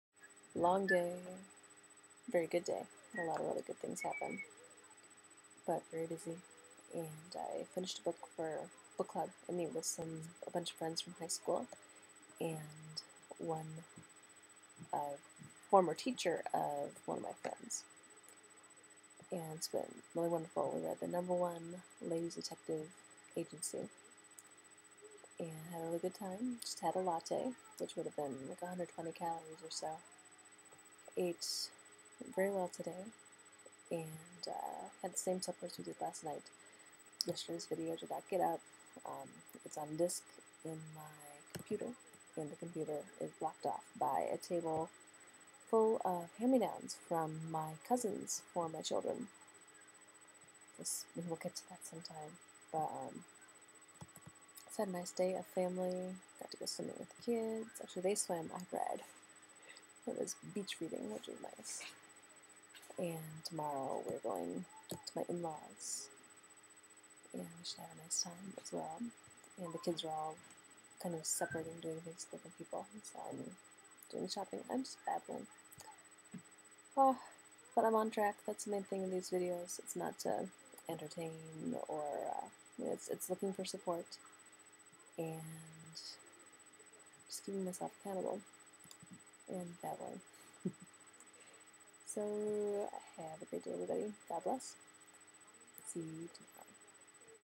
Book club and hand me downs
TIRED!
Breakfast: 3/4 c. mixed berries, 3/4 c. Greek yogurt, 2 T. sliced almonds
Lunch: Ham and cheese on whole wheat/fiber bread (mayo), beef stick, banana, 1/2 serving potato chips, 2 oz. cheddar cheese
Snack: McD's cone, McD's berry smoothie (darn, McD's again?)
Supper: Red potato, black beans, salsa, sauteed zucchini/onions/garlic, green salad w/ cucumbers (both from my garden!), tomatoes, light ranch dressing
Snack: Nonfat latte